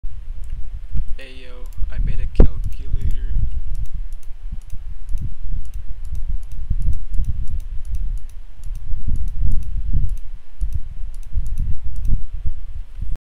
guys guys i made a calculator
right now, its just like 1 + 1 = 2
each lever adds one
the OR gates are basically the LEDs